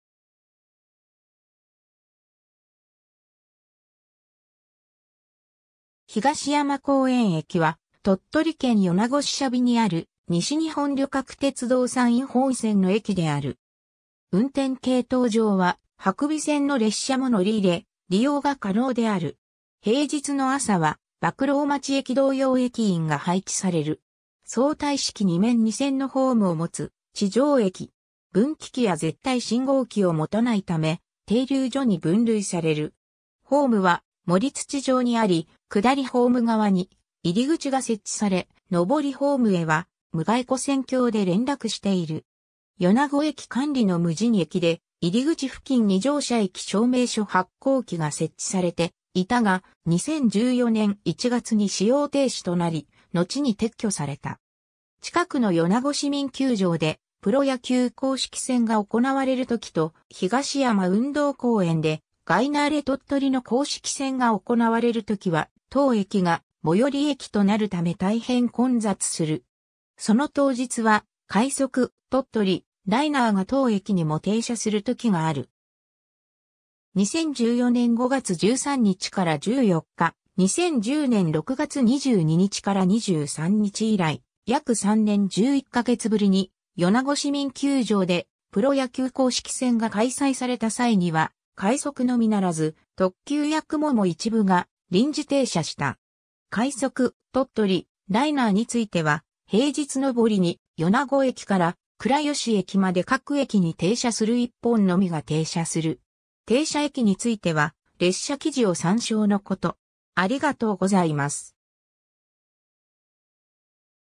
東山公園駅 (鳥取県)
東山公園駅（ひがしやまこうえんえき）は、鳥取県米子市車尾（くずも）にある西日本旅客鉄道（JR西日本）山陰本線の駅である。
運転系統上は伯備線の列車も乗り入れ、利用が可能である。
 平日の朝は博労町駅同様駅員が配置される。
 相対式2面2線のホームを持つ地上駅。
ホームは盛土上にあり、下りホーム（2番のりば）側に入口が設置され、上りホーム（1番のりば）へは無蓋跨線橋で連絡している。
米子駅管理の無人駅で、入口付近に乗車駅証明書発行機が設置されていたが、2014年1月に使用停止となり、のちに撤去された。
 近くの米子市民球場でプロ野球公式戦が行われる時と、東山運動公園でガイナーレ鳥取の公式戦が行われる時は、当駅が最寄駅となるため大変混雑する。
その当日は、快速「とっとりライナー」が当駅にも停車する時がある。
 2014年（平成26年）5月13日 - 14日、2010年（平成22年）6月22日 - 23日以来約3年11ヶ月ぶりに米子市民球場でプロ野球公式戦が開催された際には、快速のみならず特急やくもも一部が臨時停車した。
 ※ 快速「とっとりライナー」については、平日上りに米子駅から倉吉駅まで各駅に停車する1本のみが停車する。
停車駅については列車記事を参照のこと。